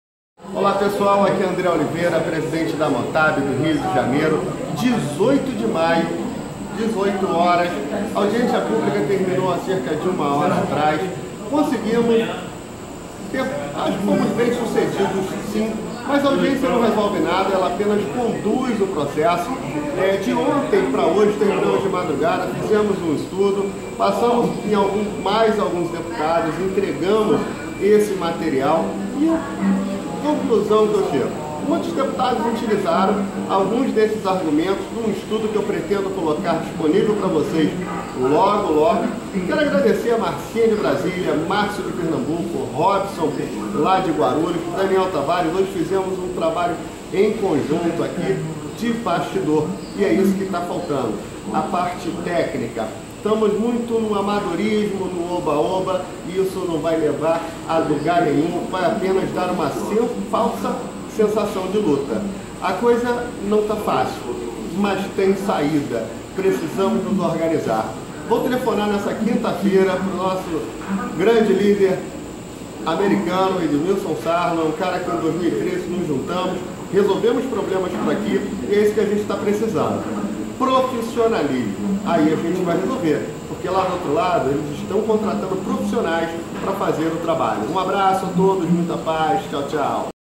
SAÍDA DA AUDIÊNCIA PÚBLICA EM BRASILIA - Tema: Transporte individual remunerado
Um recado aos colegas no final de mais uma audiência pública no debate sobre transporte individual remunerado